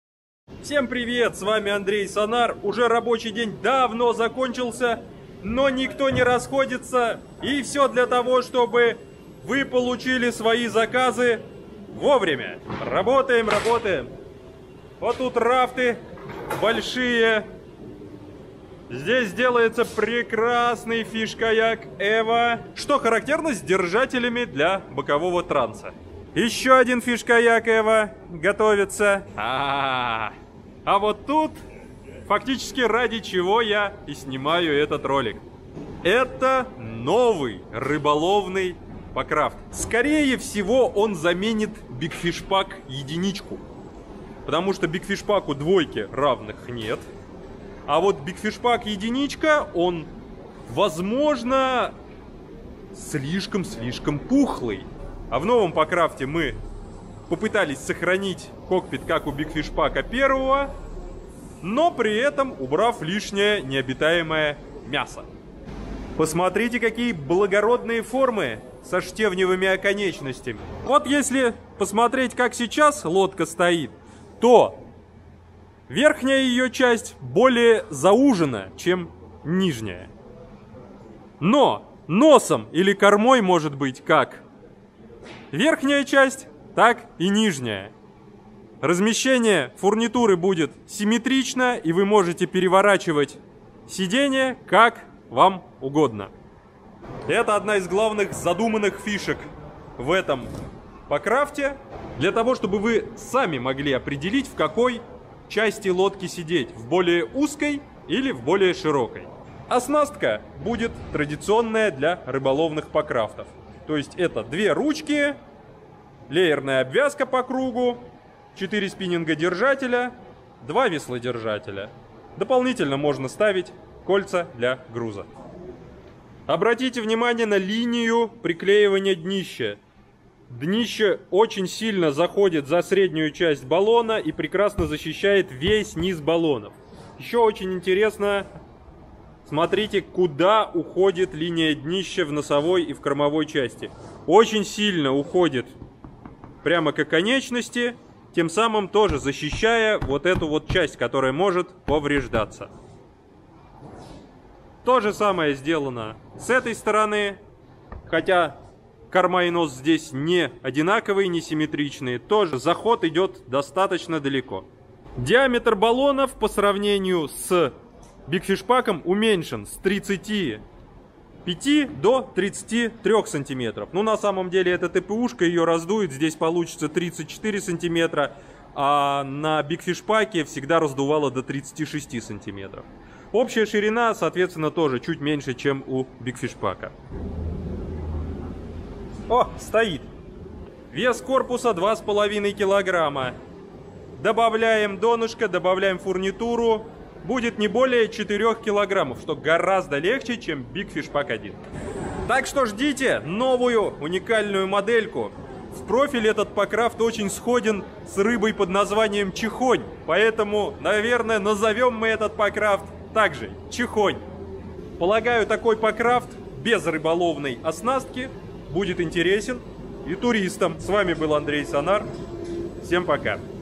В разработке: новый рыболовный пакрафт
Мы разработали новый пакрафт для рыбалки. Скоро на испытания!
Длина 345 см, ширина 106 см, диаметр баллона 33 см.

Присоединяйтесь к нам в Телеграмме - "Клуб Владельцев Лодок ТаймТриал" ! Там много полезного и интересного!

time trial #